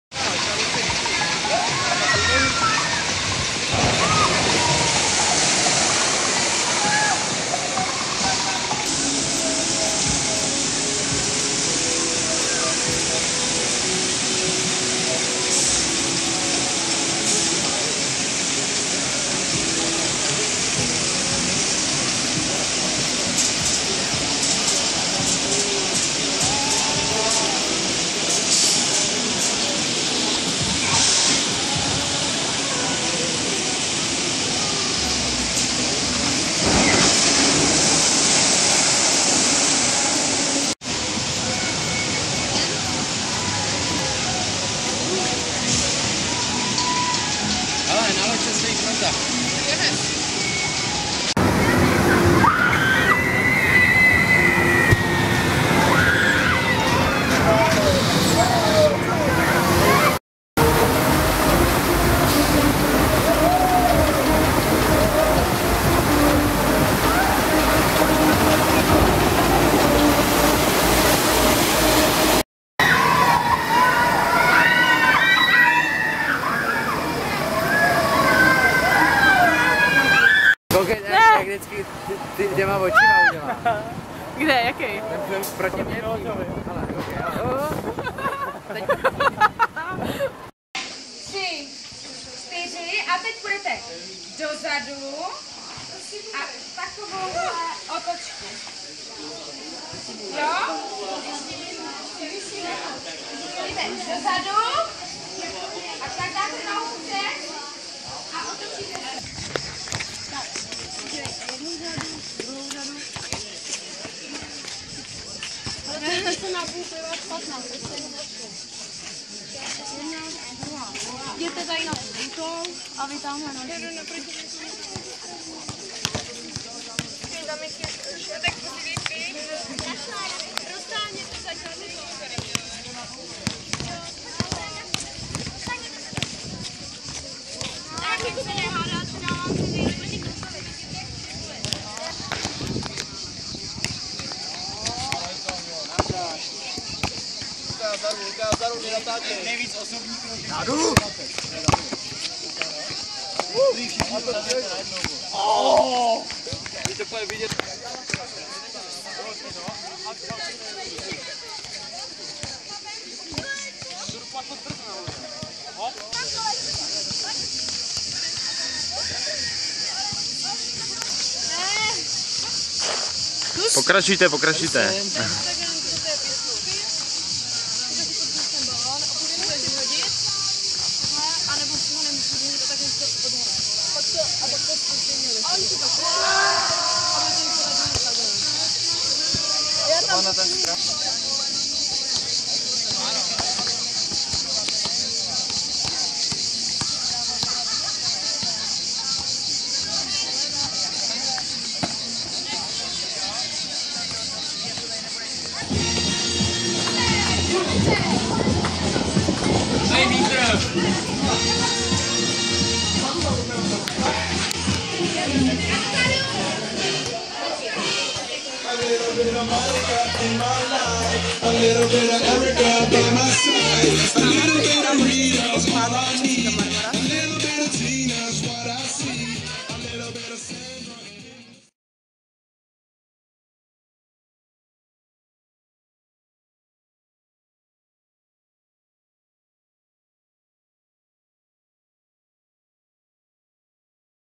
MK 2014 3.turnus Řecko - 22.7. Výlet do Magic parku a večerní kroužky
Mořský koník Řecko 3.turnus 2014